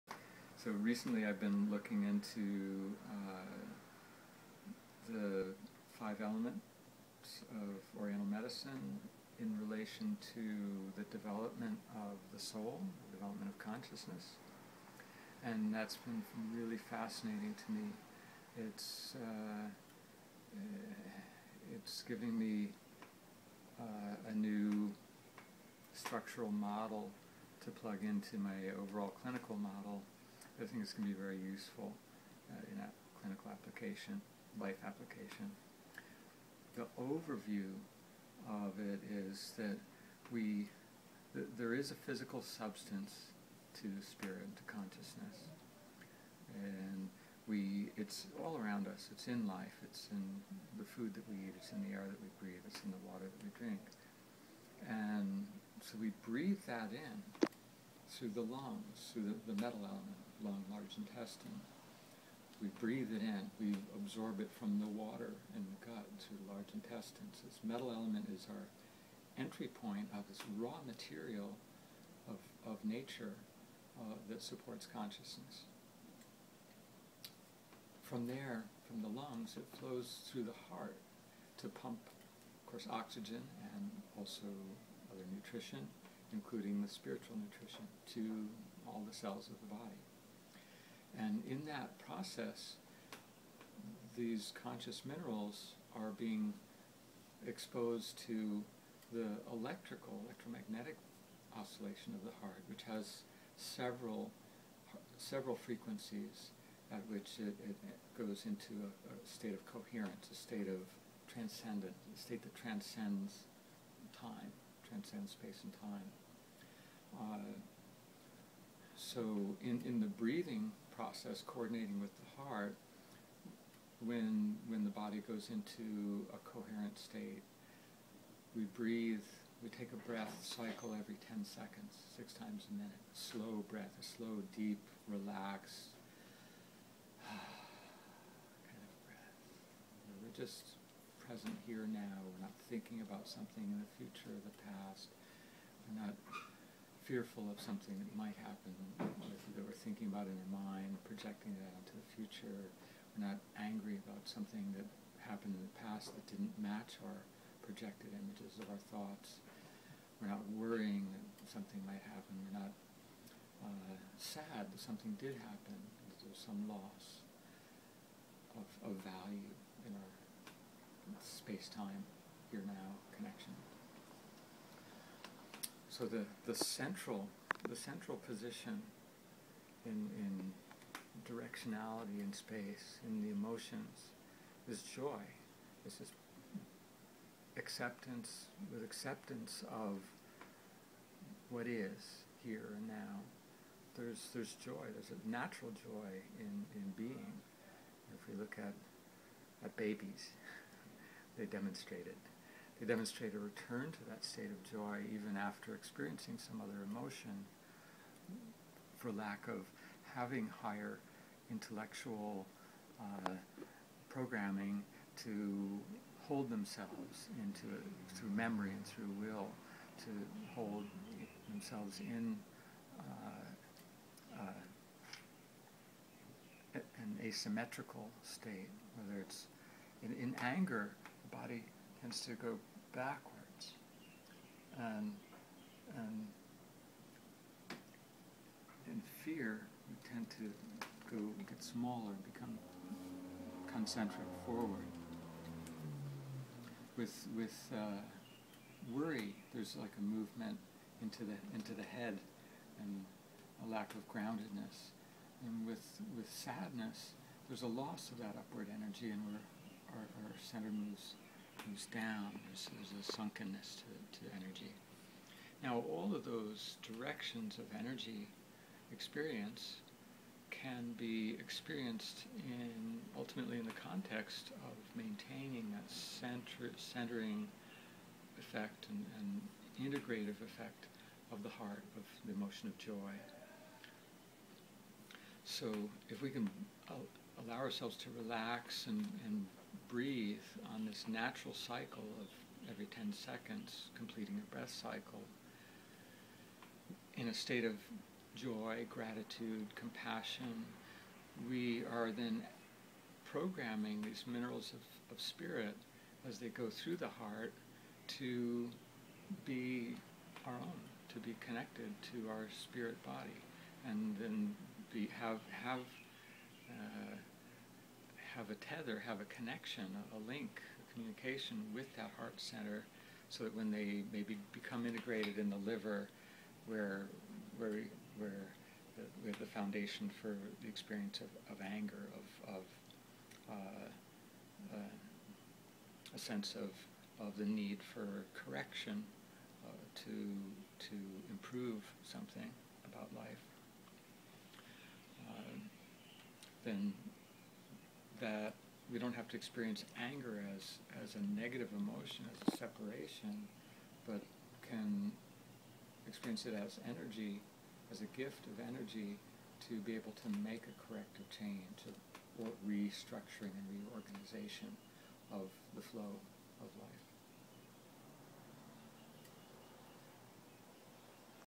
Breath & Emotions
Introduction to concepts of 5 Element Theory on breathing in the jing as substrate for consciousness, and model of emotions with directionality and joy of heart centering and integration.  Using Wood Element anger as energy of correction and reorganization.